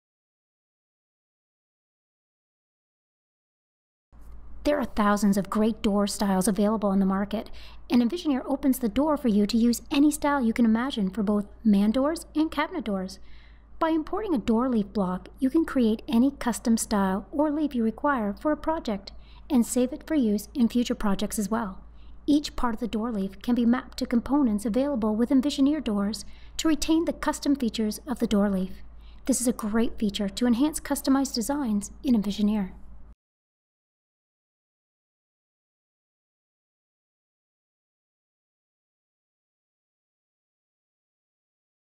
Envisioneer Customizable Door Leafs
There are thousands of great door leaf styles available on the market and Envisioneer opens the door for you to use any style you can imagine for both man doors and cabinet doors. By importing a door leaf block you can create any custom style of leaf you require for a project and save it to use in future projects as well. Each part of the door leaf can be mapped to components available with Envisioneer doors to retain the custom features of the door leaf. This is a great feature to enhance customized designs in Envisioneer.